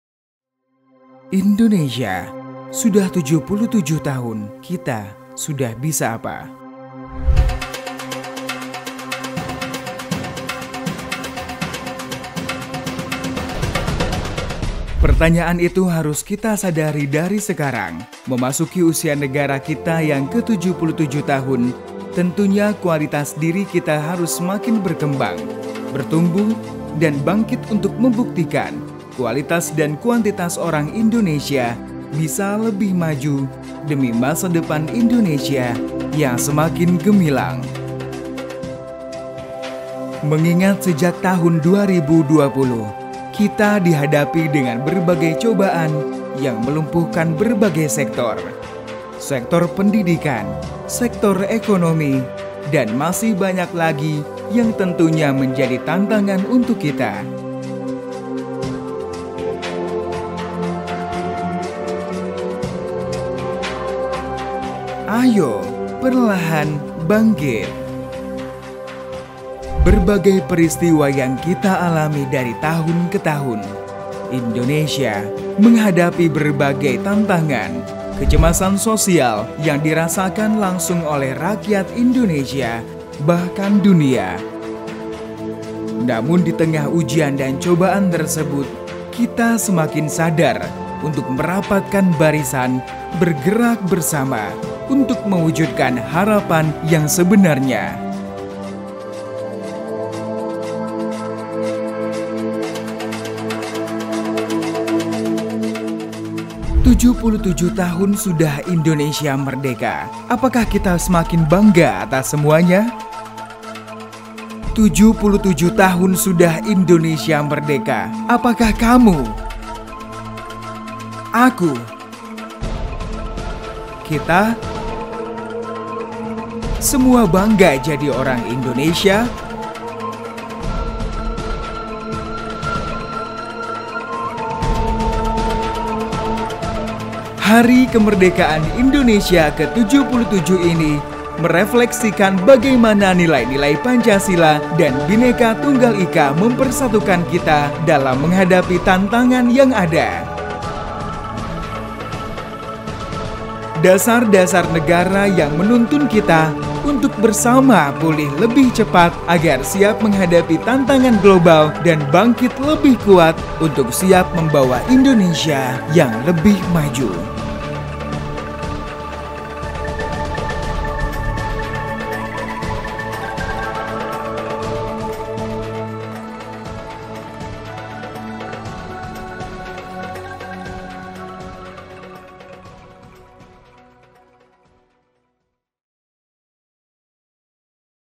Pulih Lebih Cepat Bangkit Lebih Kuat
Masih dalam semarak memperingati HUT ke-77 Kemerdekaan RI, Kementerian Sekretariat Negara (Kemensetneg) melakukan kampanye kemerdekaan ke beberapa lokasi keramaian di Jakarta. Dengan menggunakan beberapa papan yang bertuliskan mengenai kemerdekaan. Hal tersebut dilakukan dalam upaya untuk memberikan informasi dan edukasi kepada masyarakat untuk memaknai arti kemerdekaan bagi mereka.

Dalam kampanye tersebut, respon dari masyarakat berbeda-beda dalam menanggapi setiap papan kampanye yang disampaikan, ada yang hanya melirik, namun ada pula yang hingga turut mengabadikan momen tersebut.